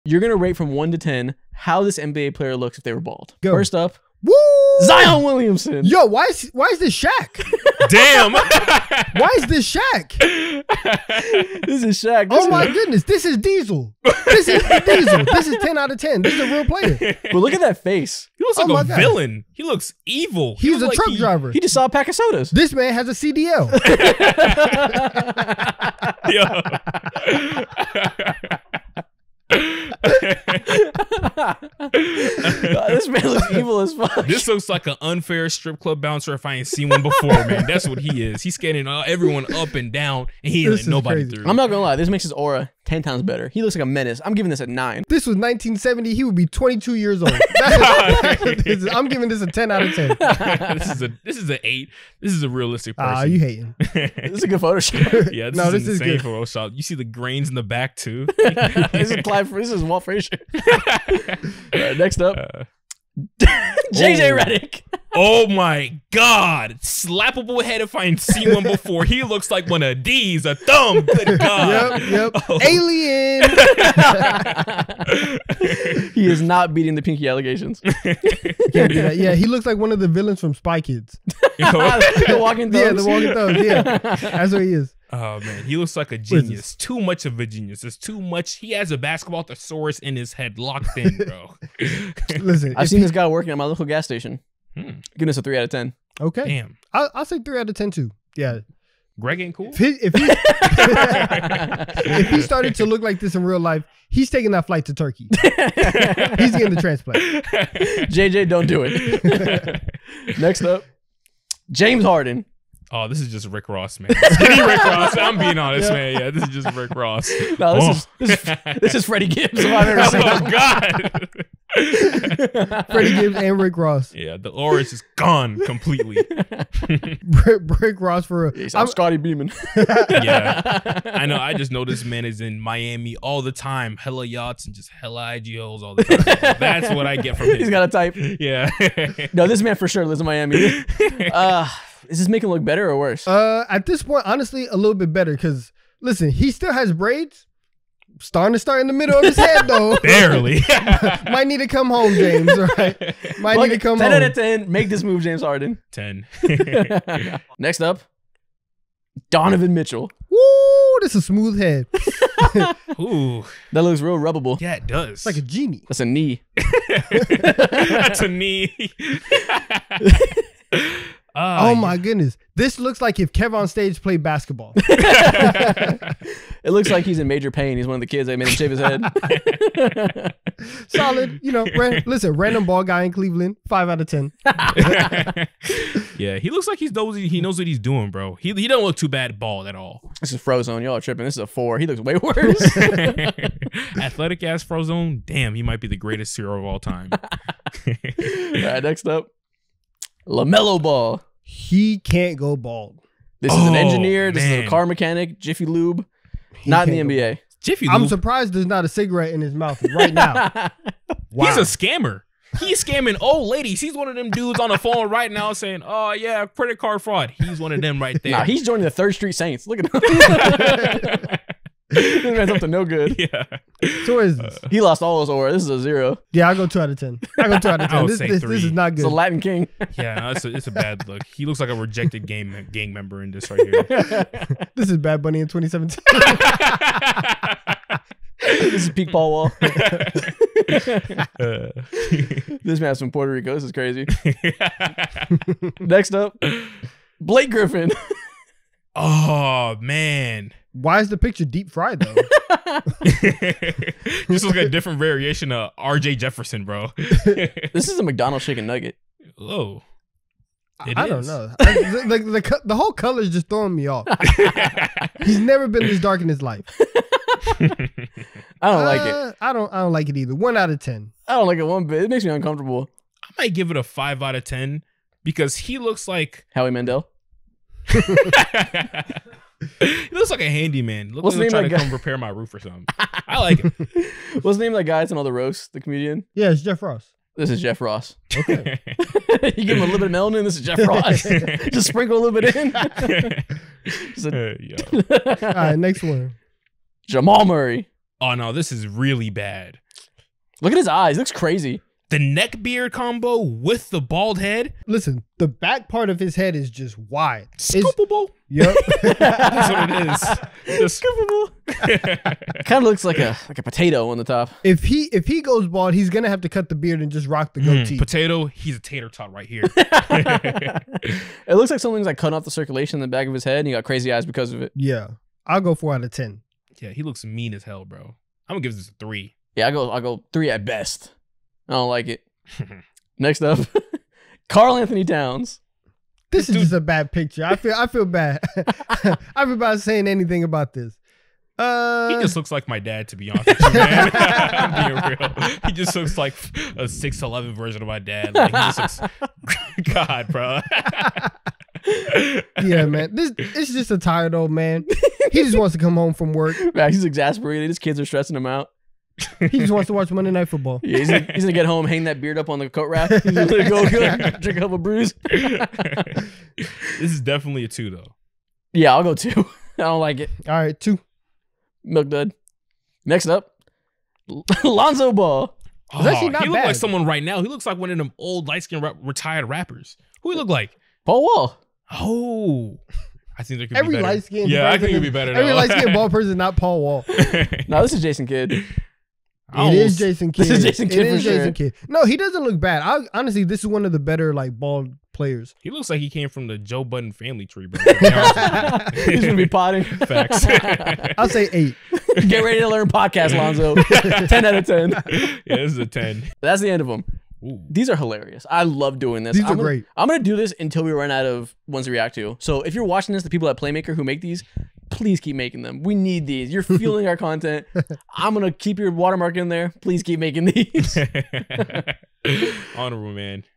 Reacting To NBA Players If They Were BALD
Today we rate NBA players if they were bald!